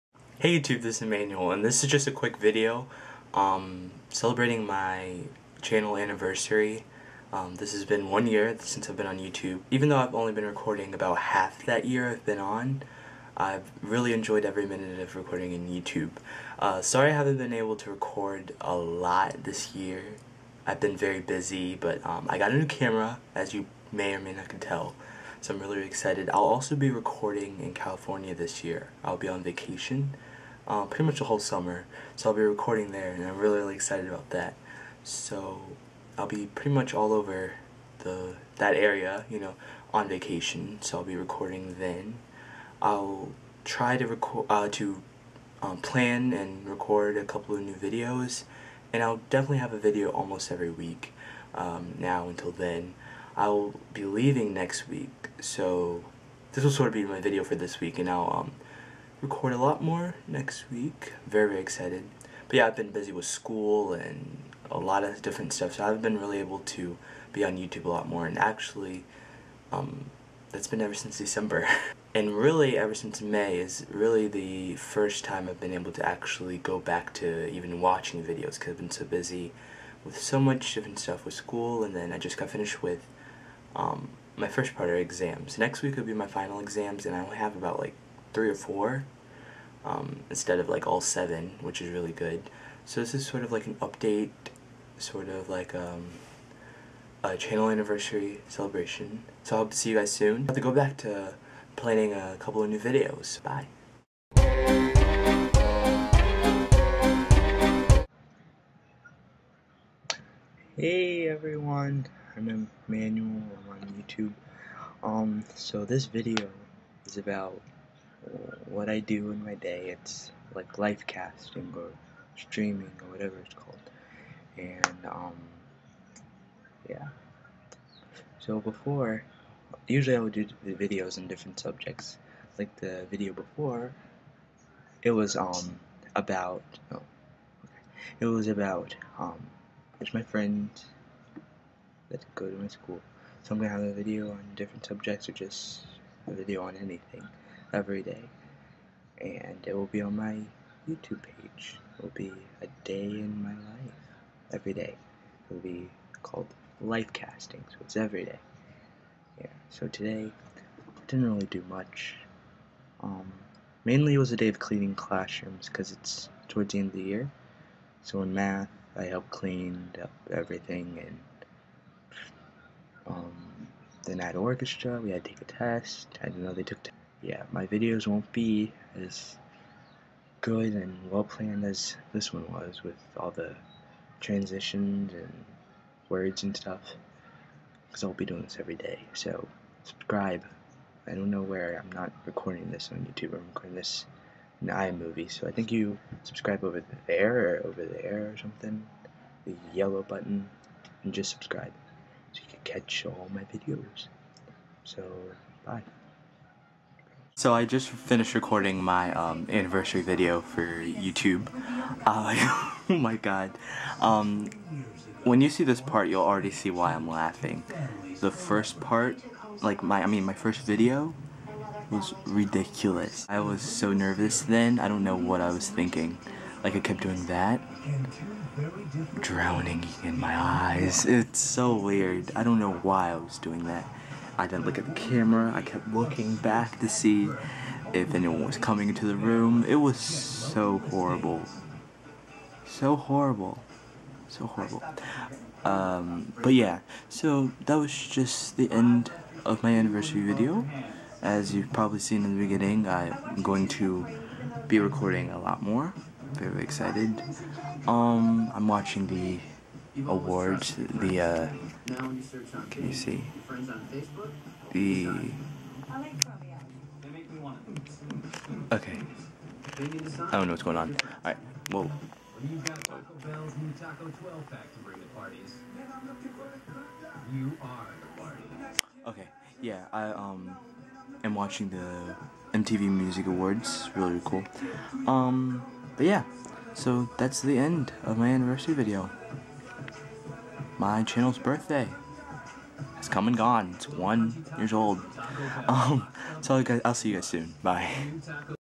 My Channel is One Year Old
This is a video celebrating the one year anniversary of being on YouTube. Now, we are doing things differently. New camera, new format, new channel year!

I'm on Instagram : therealemmanuel